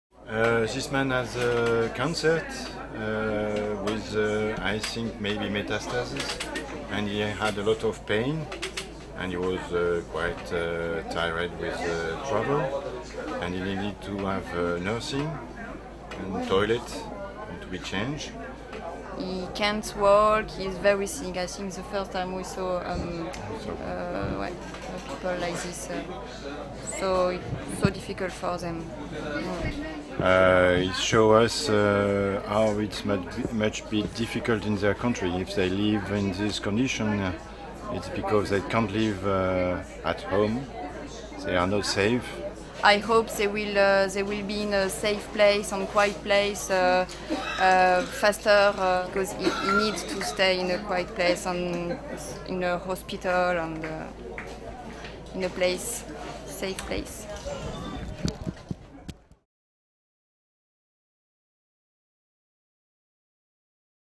- Den sykeste pasienten vi har behandlet
Mohammed fra Idlib i Syria er kreftsyk. Likevel har han og konen sett seg nødt til å flykte. De håper de kommer seg frem til Tyskland i live. - Det er første gang vi har behandlet en så syk pasient, forteller sykepleier Aurélie Gaudiche, som jobber på Røde Kors-klinikken i greske Idomeni.